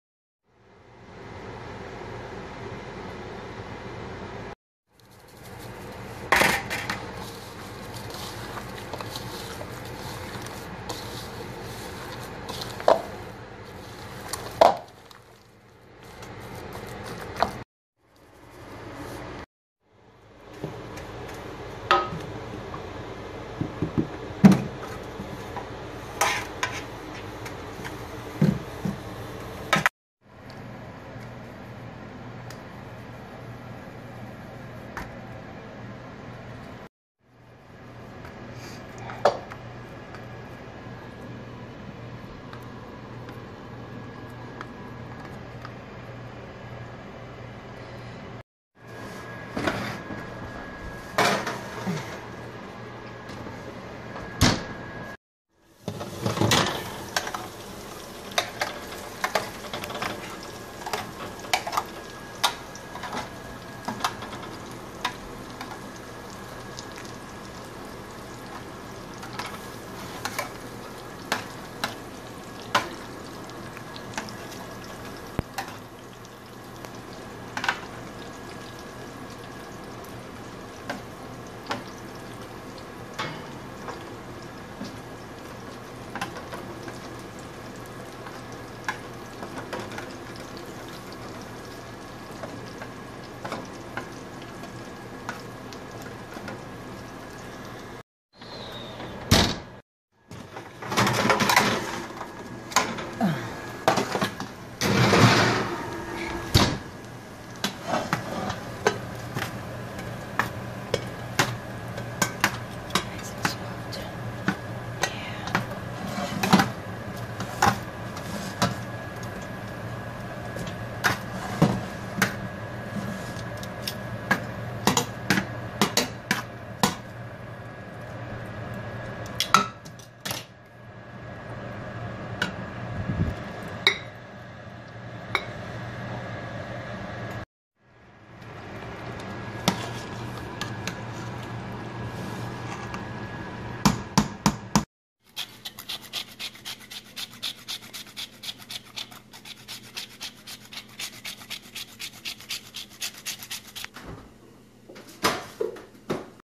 Meatballs Food Compilation #24
24th food compilation of my cooking and food review channel series on YouTube and the Internet, Cindys Home Kitchen. In this video I make some Meatballs for lunch time or dinner time. I combine the ground beef and breadcrumbs together and then put the oil in the baking pan and pop them in the oven, no frying here!